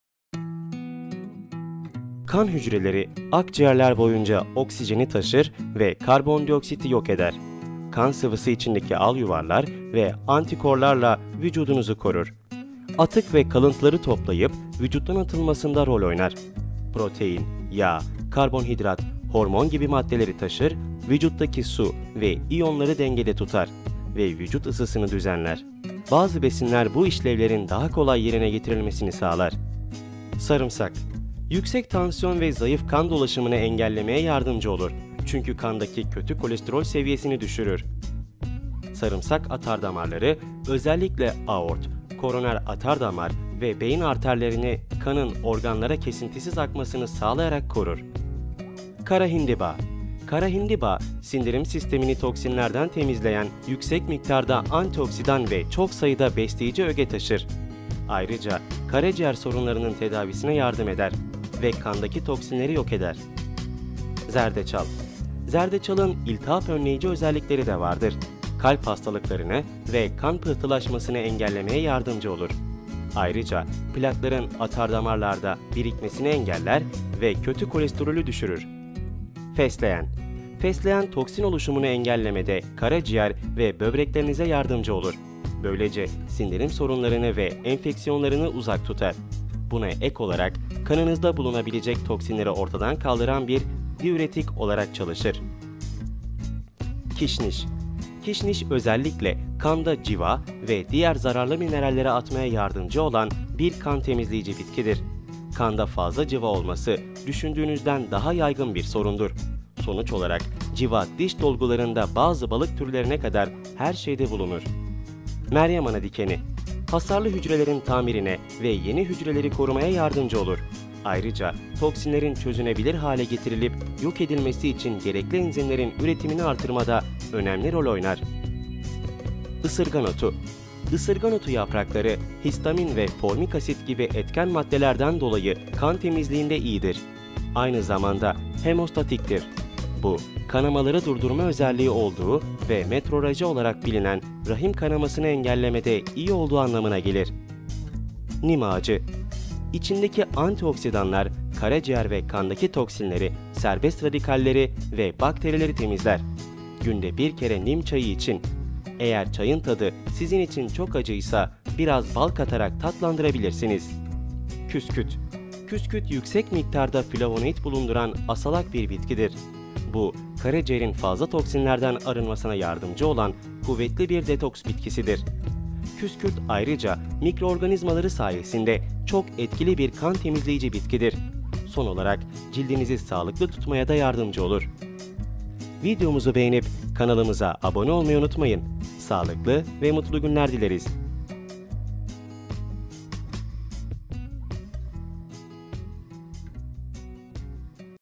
Bu bitkiler Kanı Temizliyor
Kanı temizleyen 9 bitki ve baharat